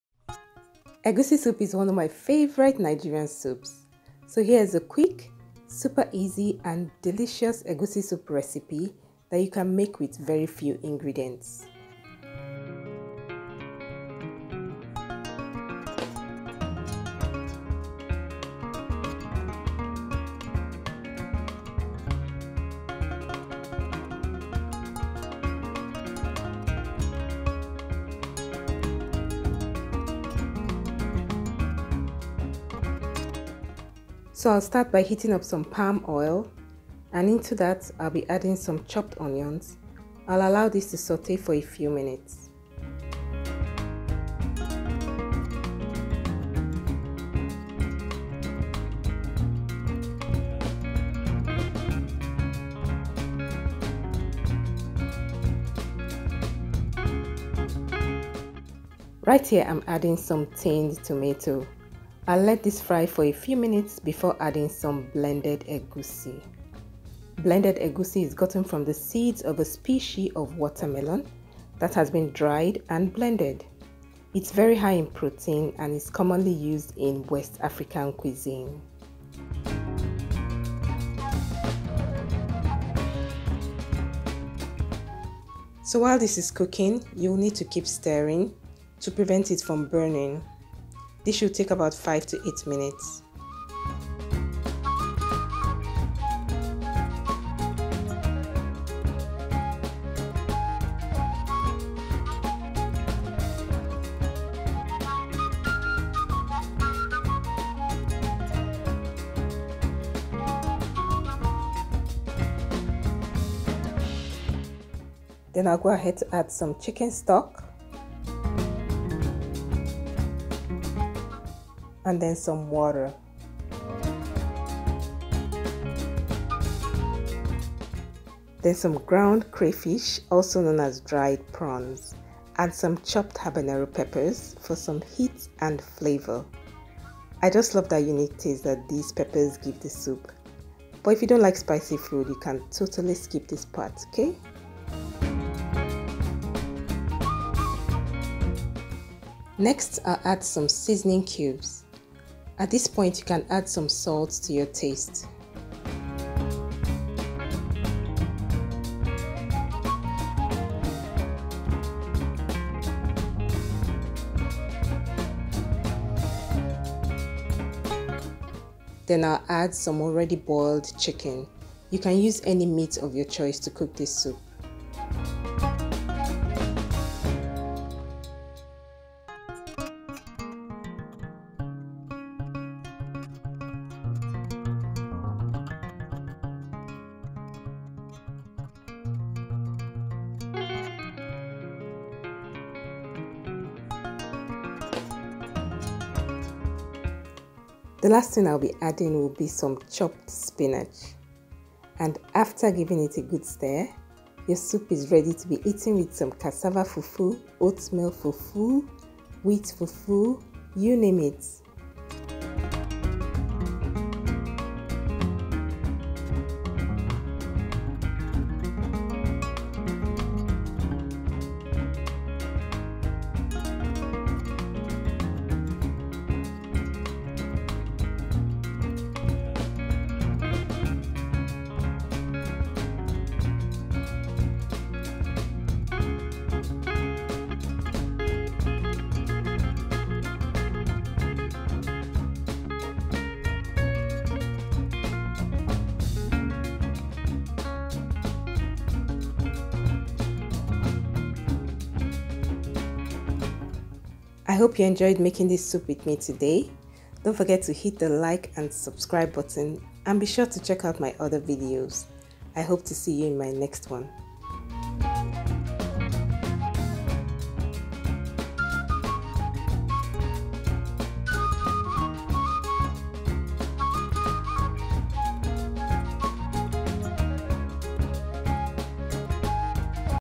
Simplified & Super Delicious Egusi Soup | Egusi Soup with Chicken & Spinach | How to Cook Egusi Soup
Hi friends! In this video, I'll show you a quick and easy method to cook egusi soup with very few ingredients.
Egusi soup is a staple dish in Nigeria and lots of West African countries. There are so many methods of making egusi soup but this method is simple and anyone can make it. It reduces your cooking time if you already have your meat cooked.
Egusi is the West African name for the seeds of a specie of melon. When dried and ground they become a staple ingredient in many West African dishes.
Egusi soup is a finger licking Nigerian soup, it's spicy, nutty and filled with exotic African flavors. It's one of the most popular soups prepared by most tribes in Nigeria with different variations and eaten with any fufu of your choice. It's known as Ofe Egusi in Igbo, Miyan Gushi in Hausa and Efo Elegusi in Yoruba.

INGREDIENTS
Egusi
Palm oil
Onions
Tinned tomato
Chicken
Chicken stock
Ground crayfish
Habanero peppers
Seasoning cubes
Spinach